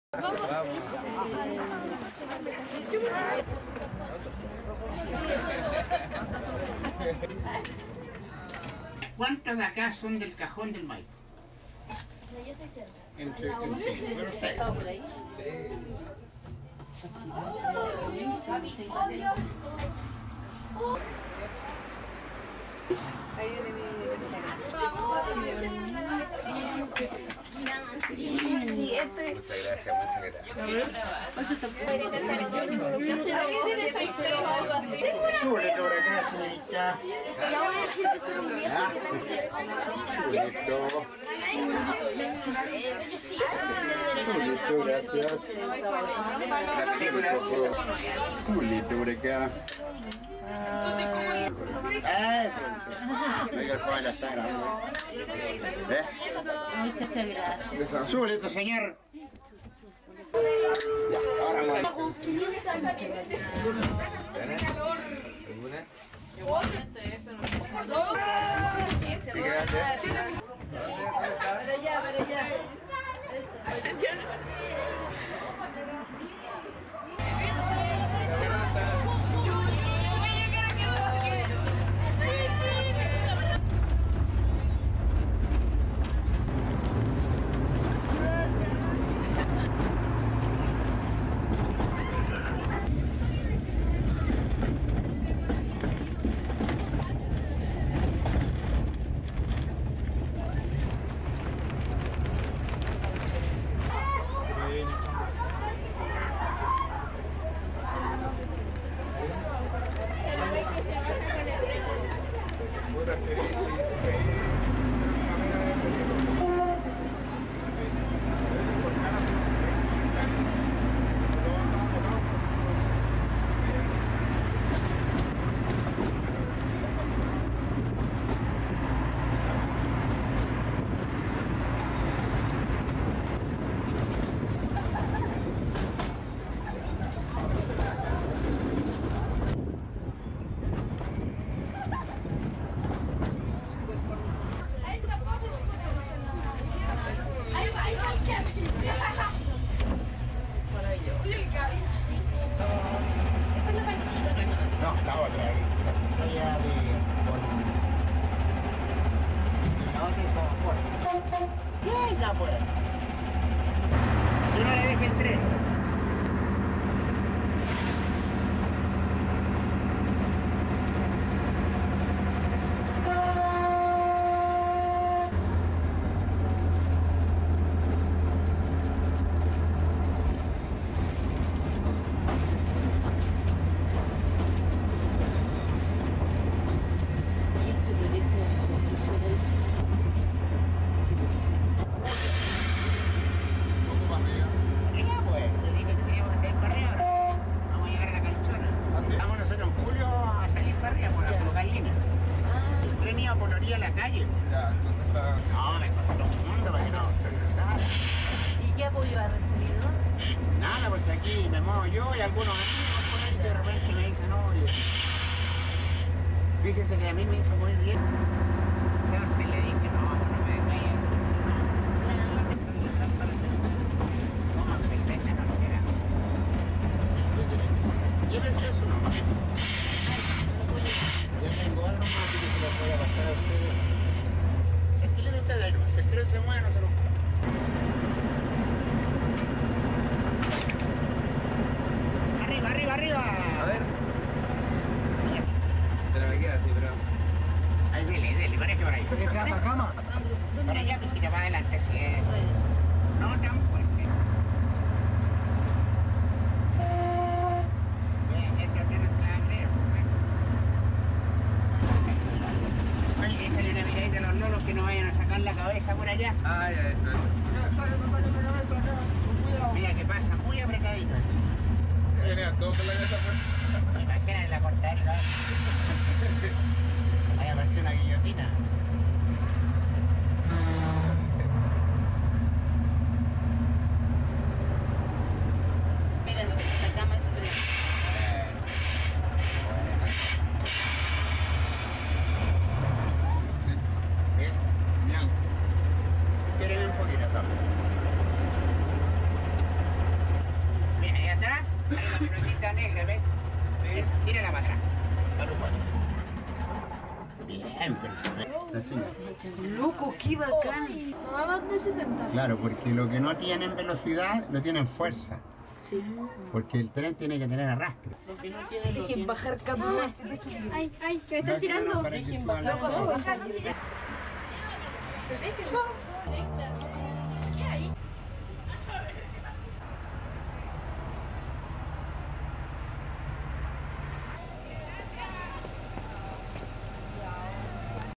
Ferrocarril Cajón del Maipo - Proyecto Ave Fénix - FFCC Puente Alto Volcán
El ferrocarril del Cajón del Maipo dejó de transitar en 1975, y luego vino el abandono y saqueo.
Pero un grupo de vecinos y ex ferroviarios liderados por Luis León revivieron el  tren que vuelve a recorrer las vías y andenes entre las montañas.
Esta fue nuestras visita el  12 abril 2019.
Gracias don Luis.